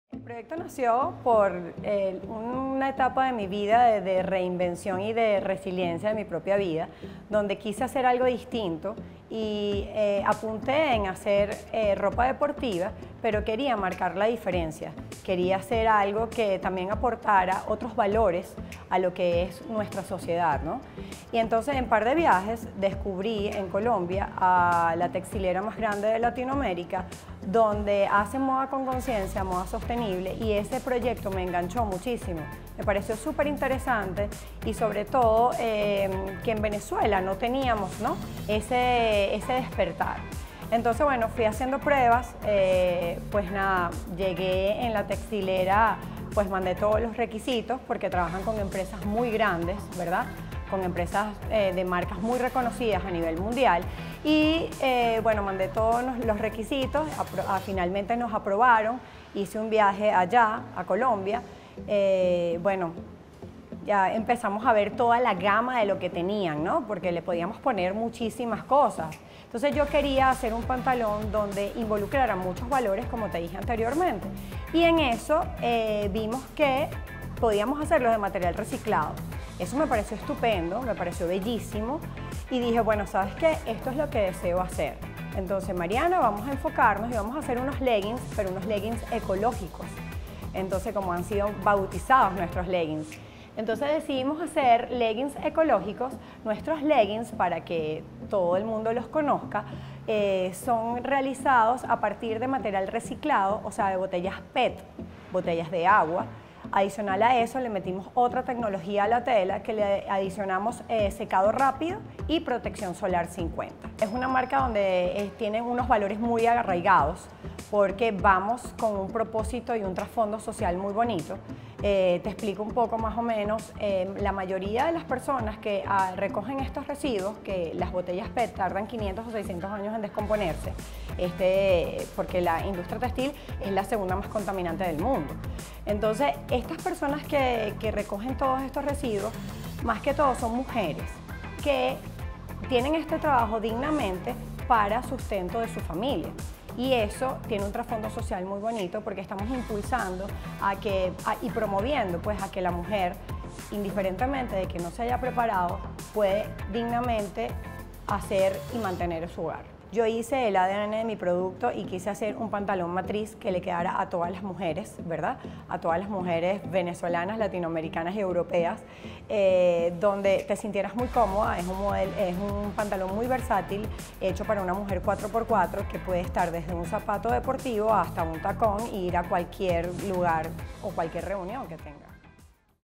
Mariana Rosales diseña los primeros leggins ecológicos de Venezuela | El Nacional
La emprendedora lanzó en julio una colección de 13 piezas hechas con botellas PEP, tecnología de secado rápido y protección solar +50

Vídeo realizado por Rose Mannuzza & Kenny Linares

Prohibida la reproducción total o parcial de cualquier material de este diario sin la autorización expresa de los editores